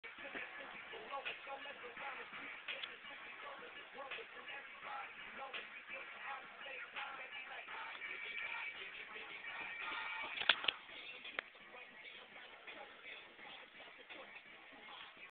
Smokin out of the Minature Homemade Steamroller!
Lil steamroller badass hit wit kottonmouth kings goin in the background!